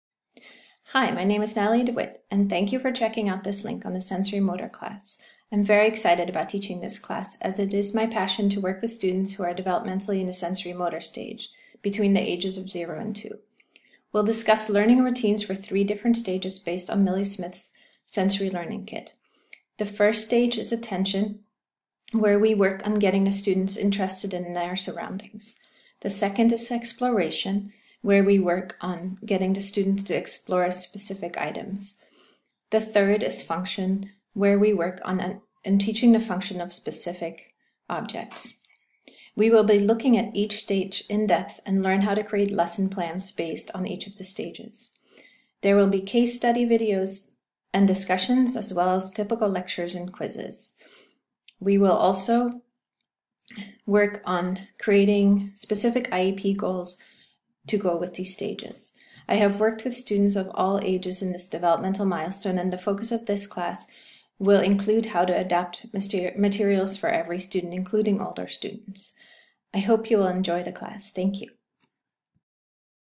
Strategies for Assessing and Developing the Learners Sensorimotor Abilities
In this online class with Nathalie de Wit, participants will learn about Sensorimotor Stage of Development, the Van Dijk Approach, levels of learning in the Sensory Learning Kit and more.

Participants will demonstrate an understanding of the unique needs of the students with visual impairments and multiple disabilities at the sensorimotor level of development and how these students can be taught meaningful routines which become a foundation for learning new concepts.